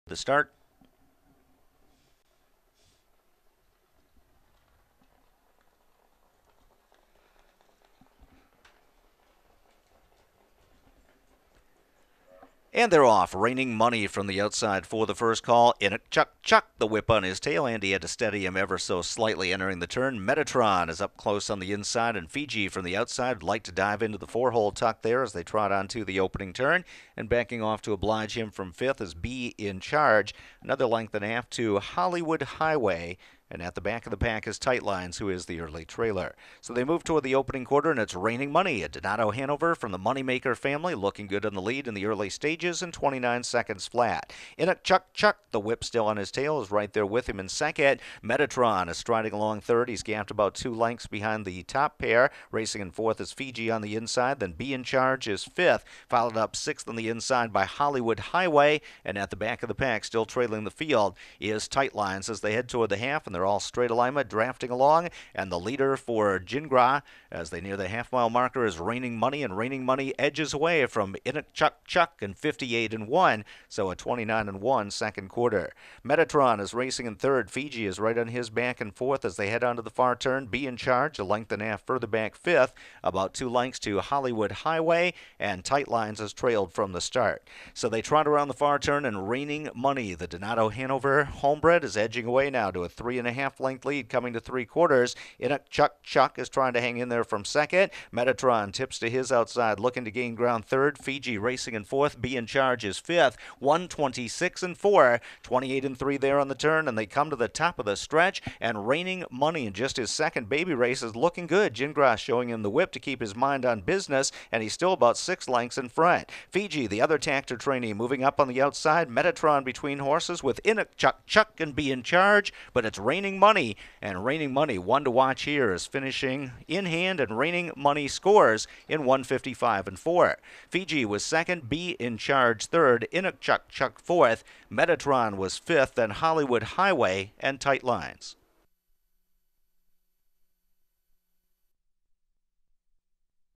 Meadowlands Baby Race #6 - July 31, 2015 - Reigning Moni
2 Year Old Colts & Geldings - 1:55.4 - Driver: Yannick Gingras - Trainer: Jimmy Takter
2nd: Fiji 3rd: Bee In Charge